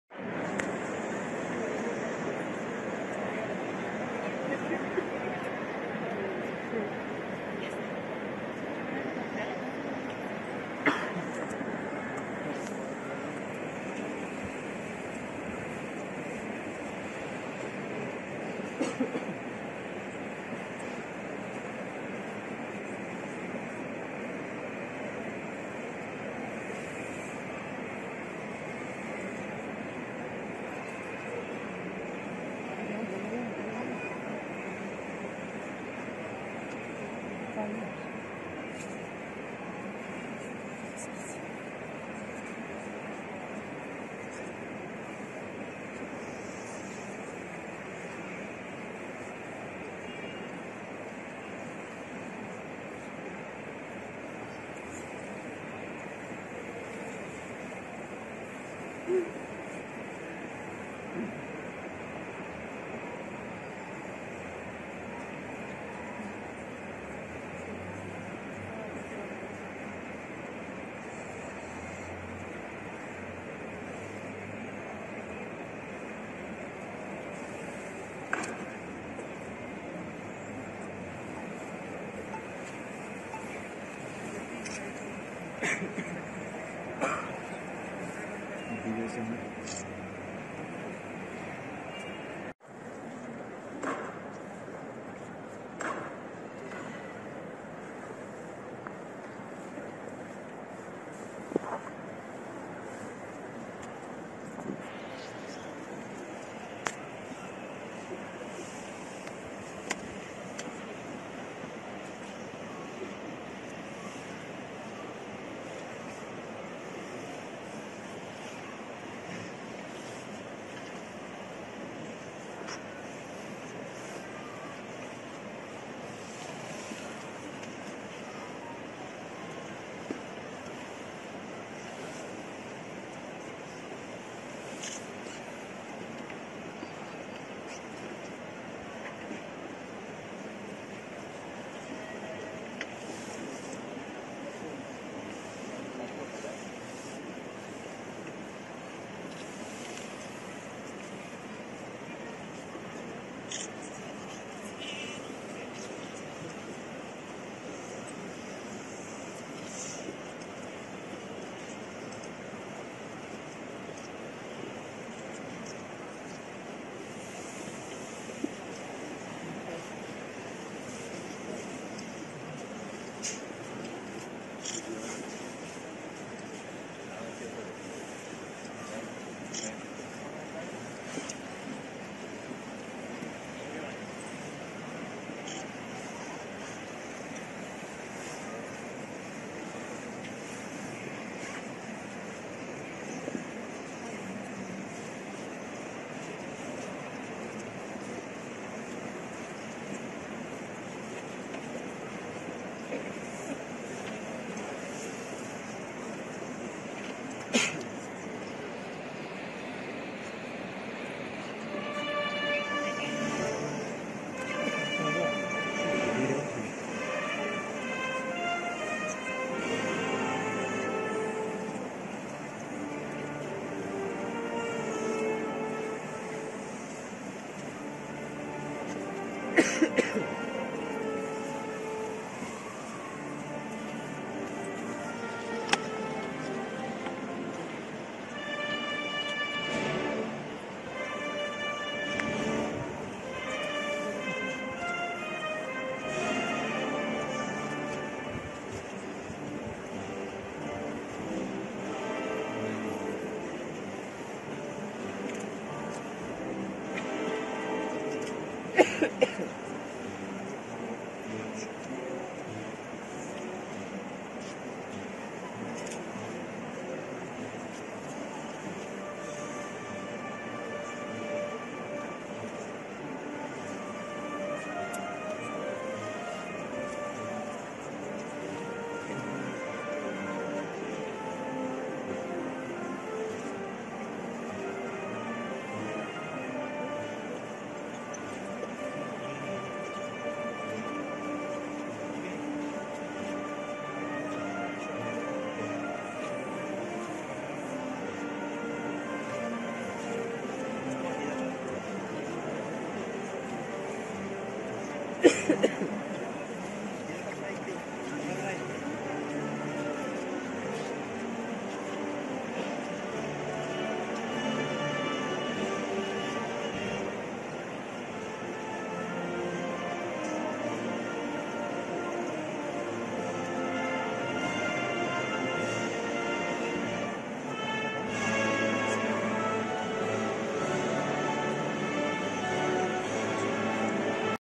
🟣 Procesión de Regreso Gran Poder - Avd. Constitución Giro Ayuntamiento - Sevilla 2021
Salida extraordinaria de Jesús del Gran Poder,  de Sevilla.

💒 Basílica de Jesús del Gran Poder, Sevilla